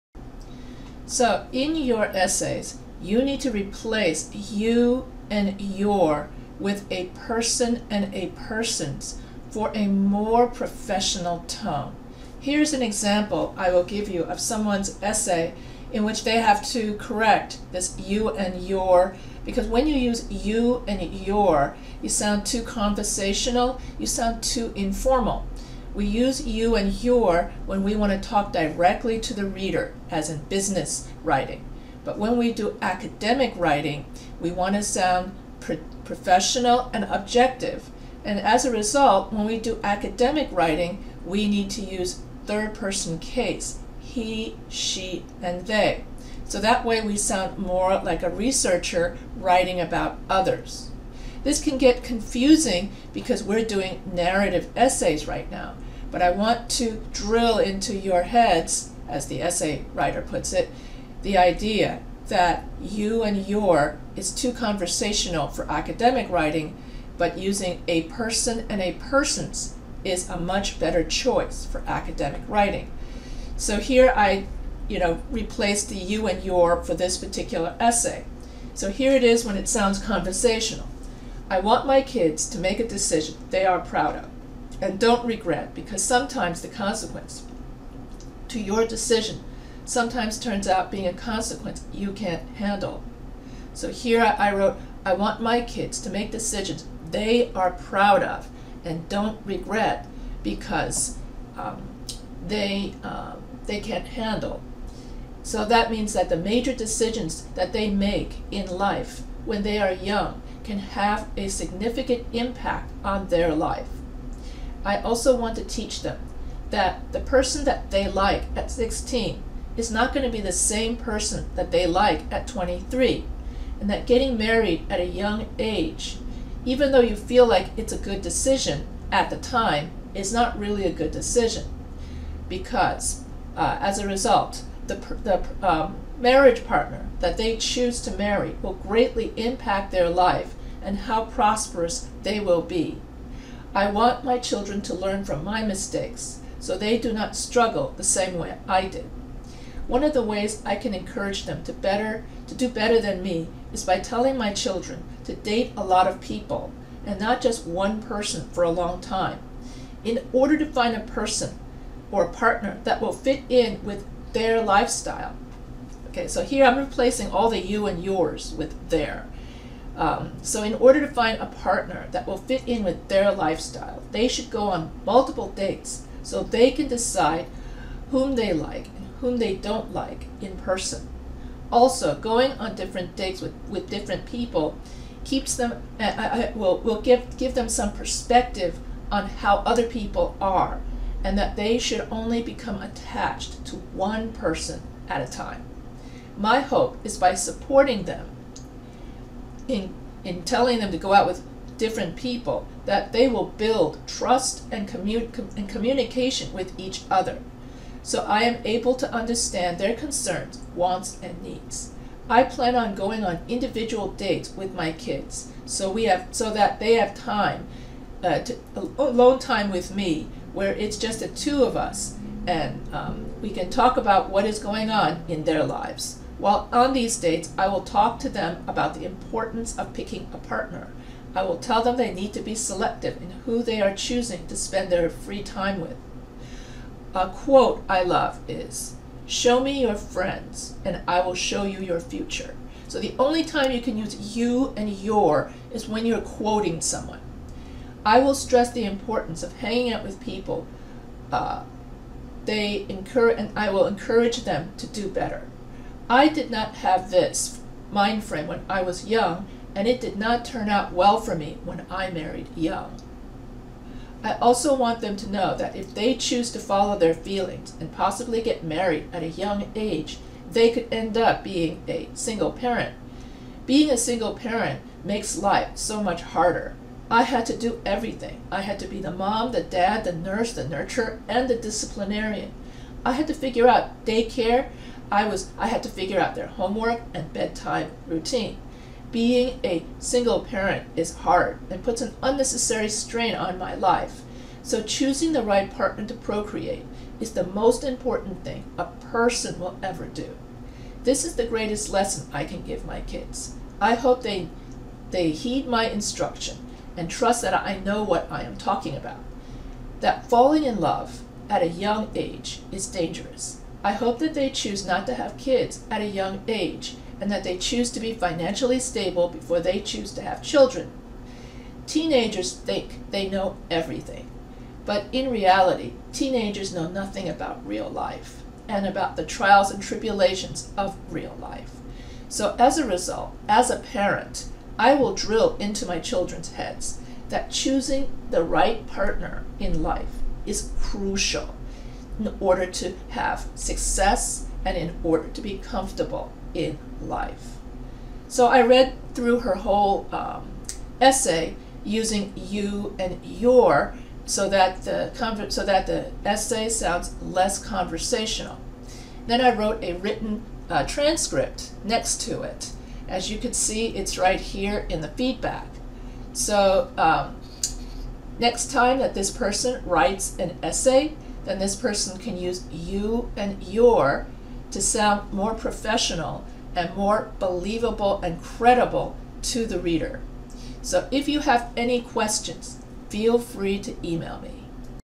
You and Your Example Paper
This video teaches you how to replace you and your with a person and a person's for a more professional tone in academic writing.